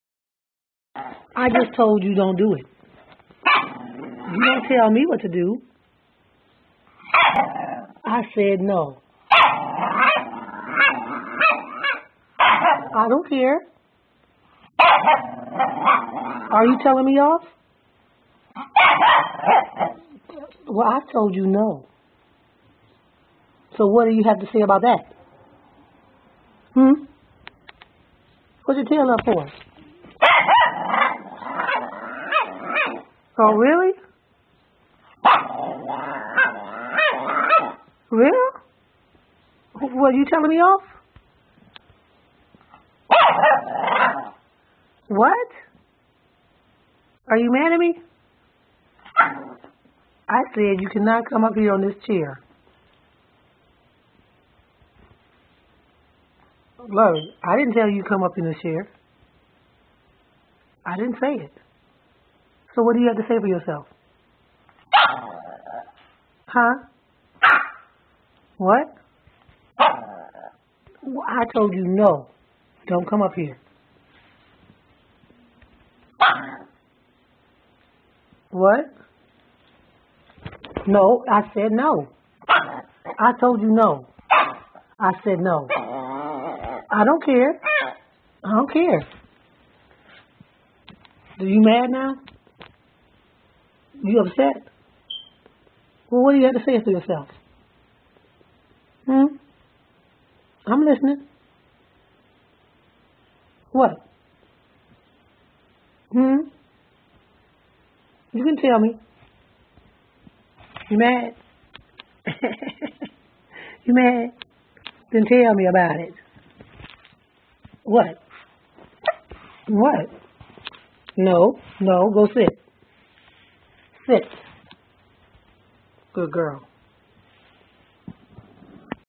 Qik - lovey telling me off i wont let her get on the chair by Pia Finley Hoffman
Streamed by Pia Finley Hoffman in United States. Qik is the fastest way to upload videos to YouTube from your mobile phone.